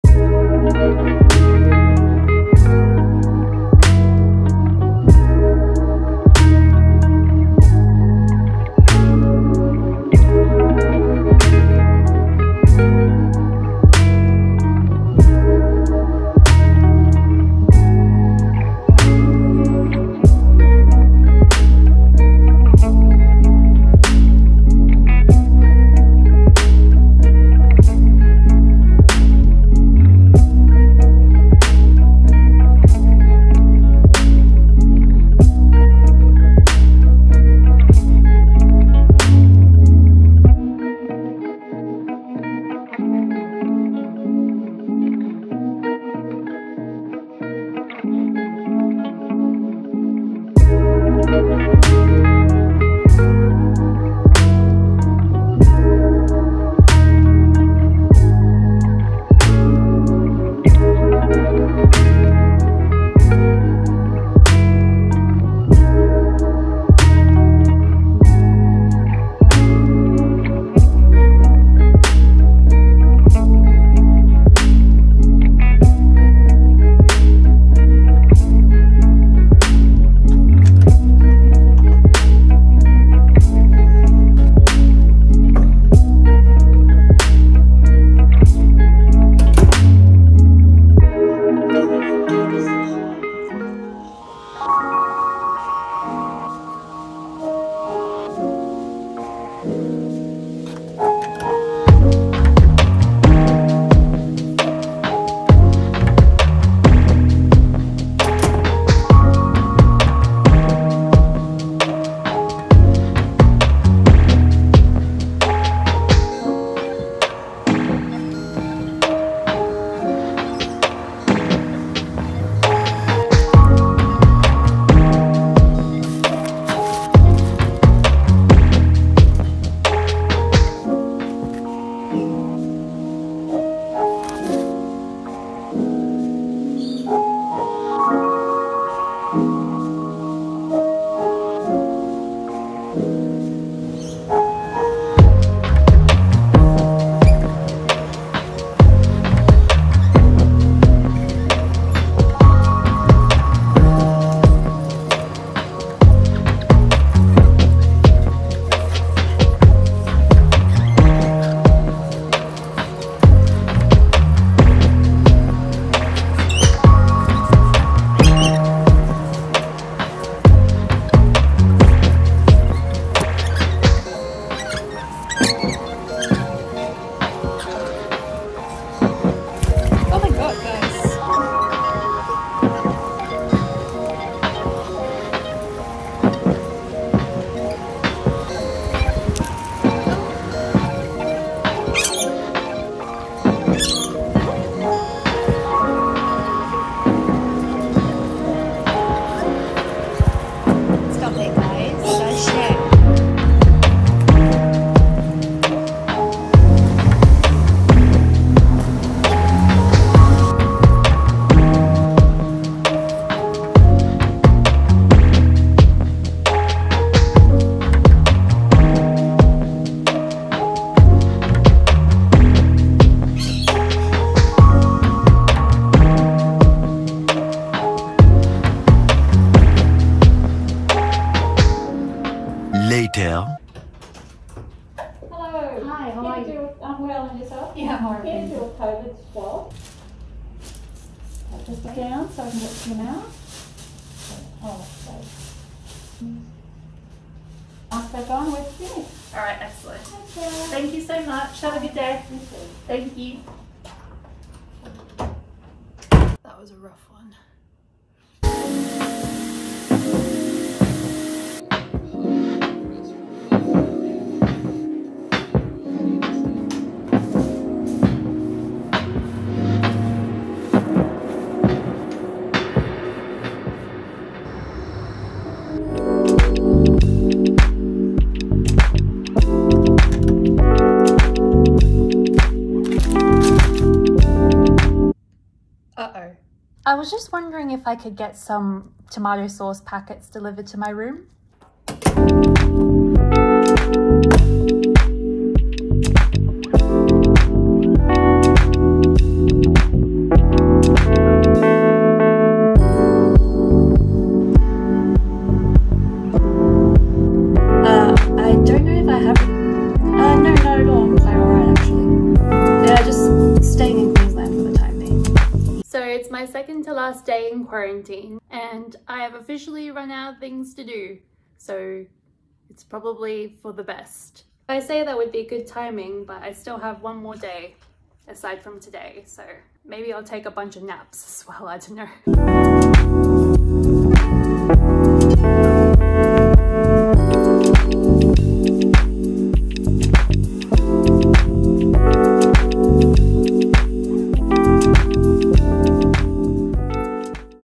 MY QUARANTINE DIARY: Day 10-12 | Results from my PCR tests in quarantine | I am now a bird mum
I received negative results for both of my PCR tests, which means I'm all set to leave quarantine in two days time. And not a moment too soon. Given another week, I would look like the bird lady from Home Alone 2. This is a video diary of Days 10 to 12 of my quarantine.

Stay safe out there, wherever you are.

S.